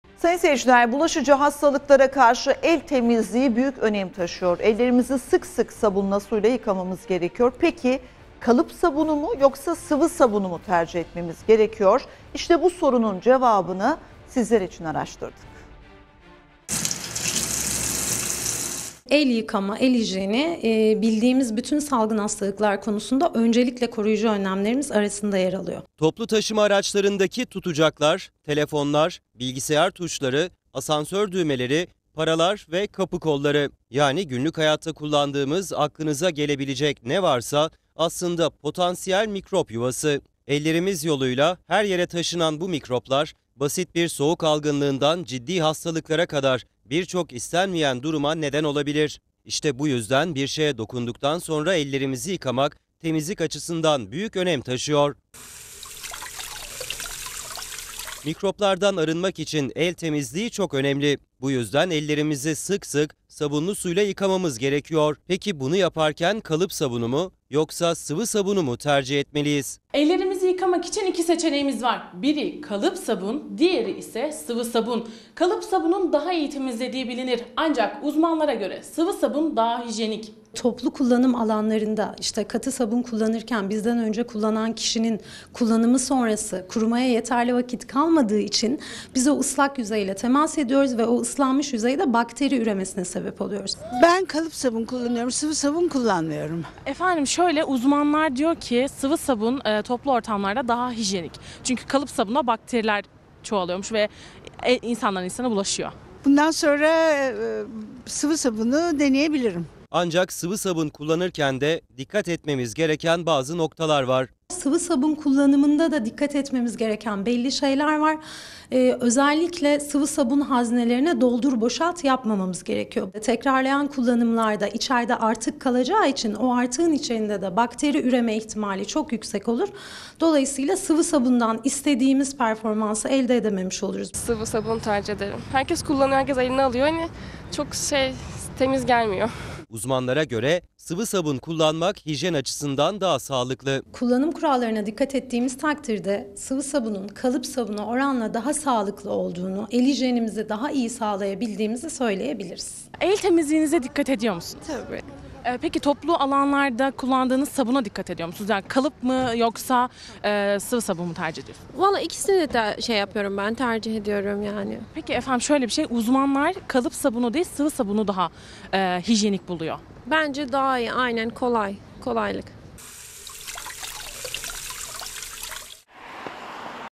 El Temizliğinde Hangi Sabun Türünü Tercih Etmeliyiz?
Bulaşıcı hastalıklara karşı el temizliği büyük önem taşıyor.. Ellerimizi sık sık sabunlu suyla yıkamamız gerekiyor..

Peki kalıp sabunu mu yoksa sıvı sabunu mu tercih etmeliyiz? Bu sorunun cevabını sizler için araştırdık..